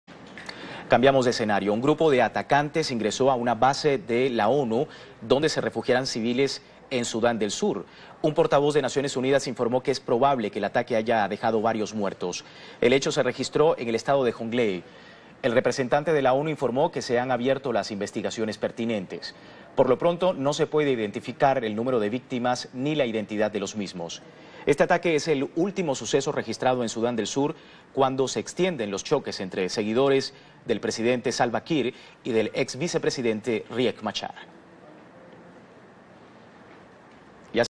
UN base in South Sudan attacked as violence spreads across the country
A United Nations base in South Sudan was attacked by an armed group, inside the place there were civilians fleeing violence that has spread across the country. teleSUR